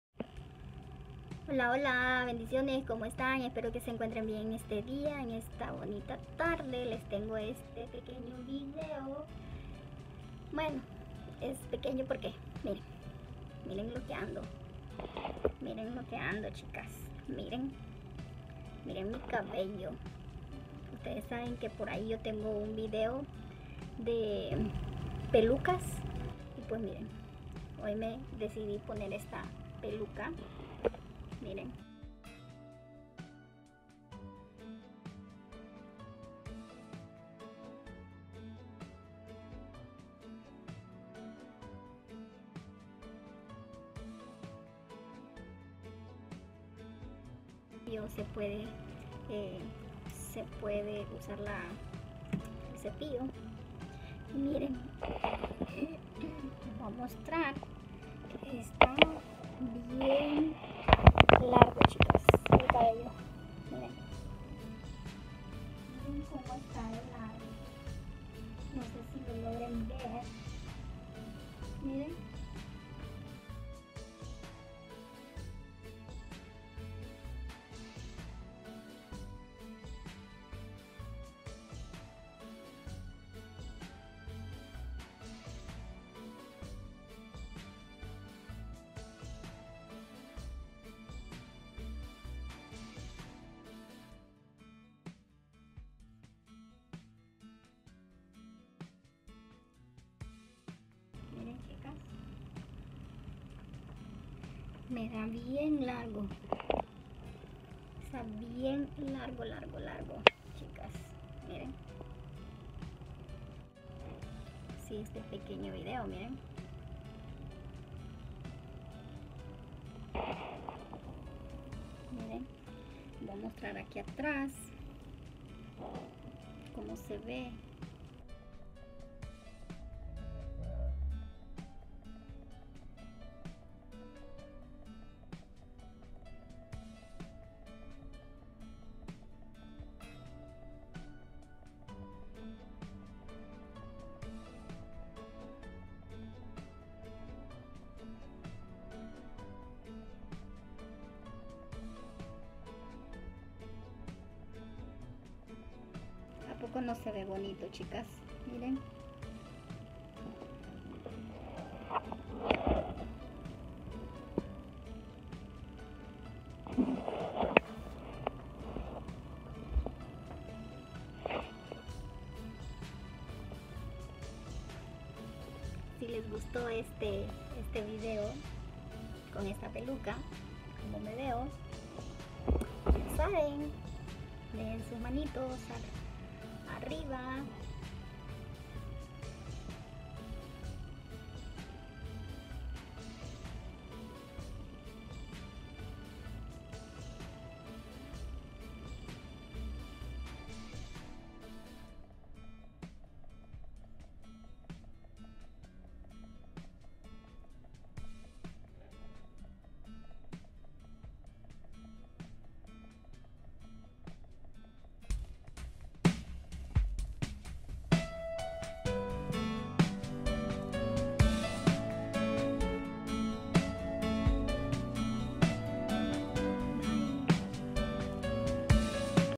Que opinan de esta peluca les gusta
si les gusta les muestro las demas que tengo como pueden ver tengo una coleccion de pelucas bueno bendiciones